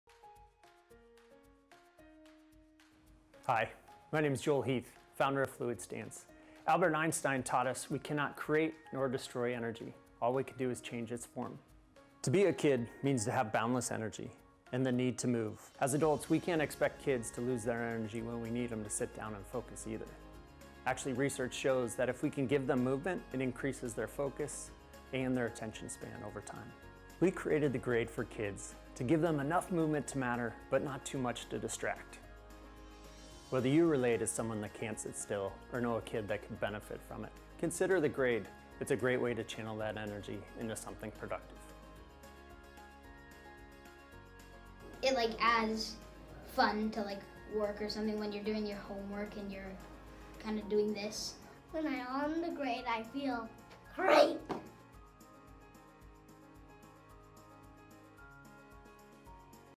The Grade for Kids Balance Board
The Grade® for kids balance board is for active kids who need to keep moving when they're doing their homework, gaming, reading, eating, and more.